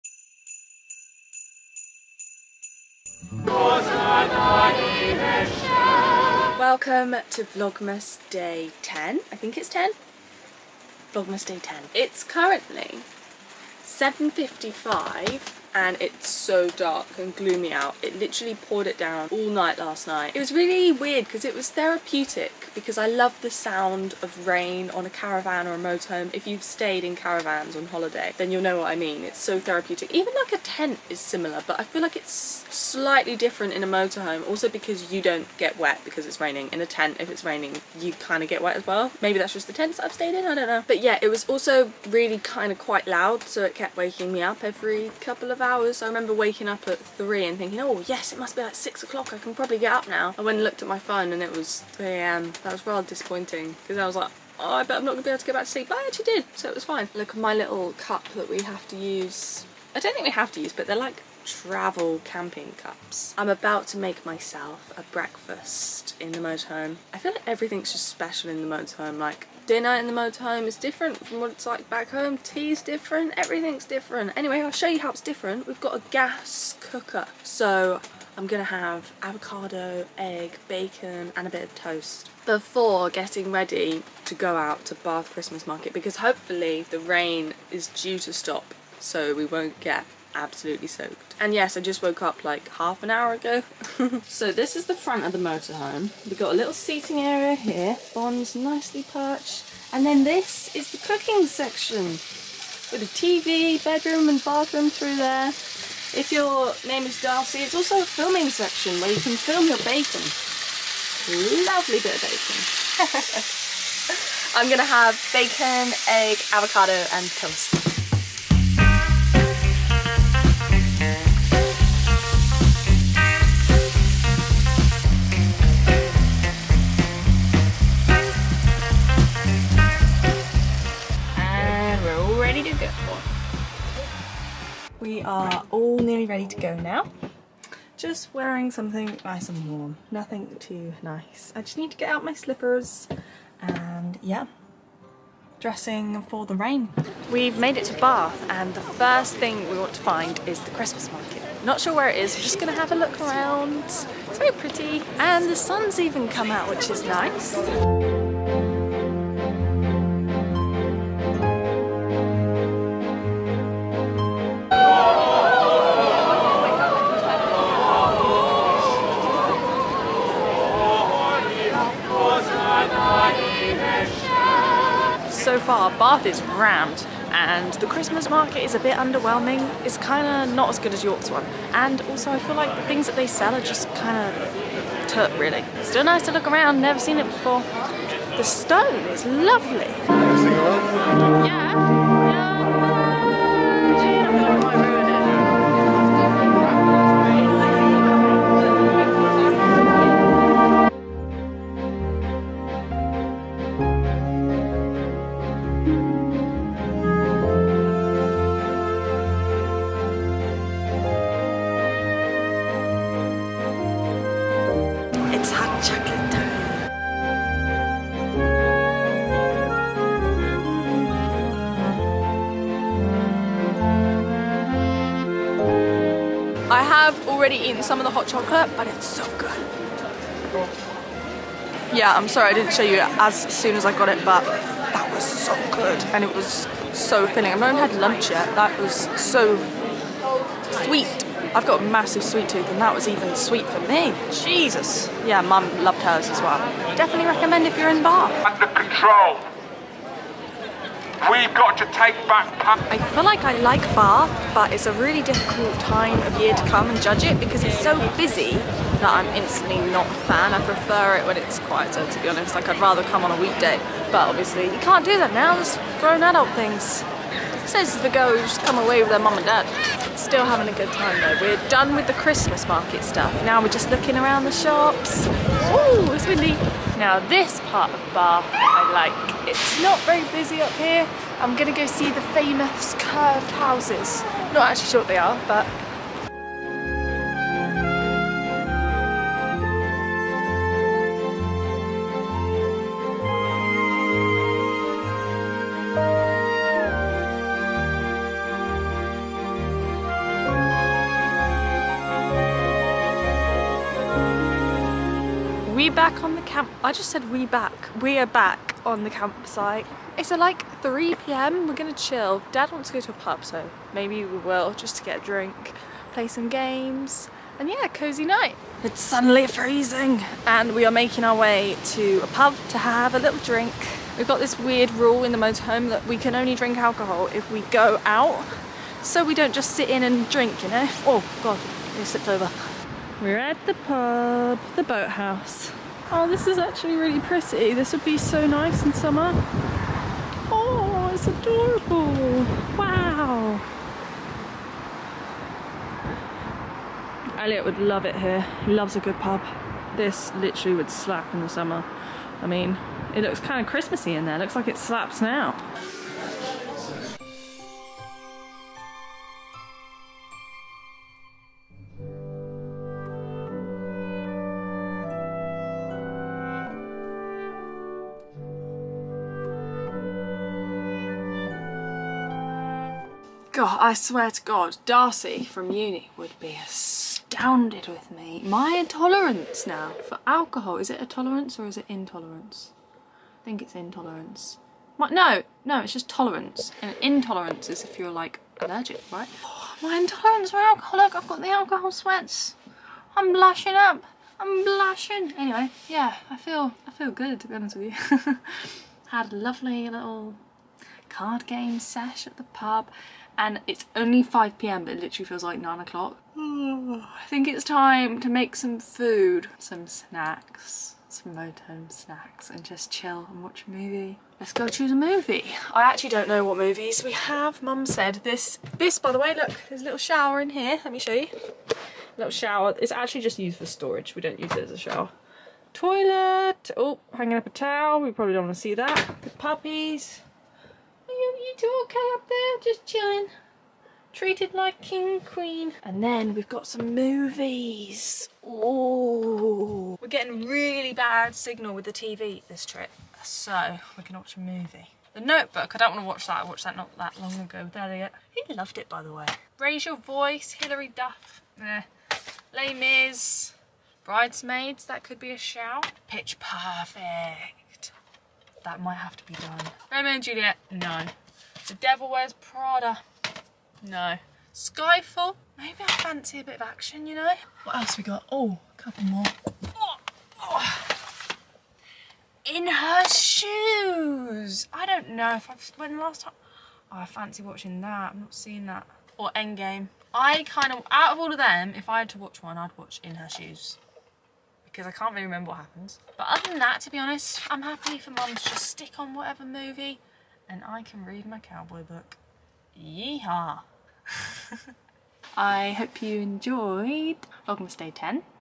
Exploring Bath Christmas Market & lots of festive food | Vlogmas day 10
That was hands down the BEST hot chocolate I've ever had!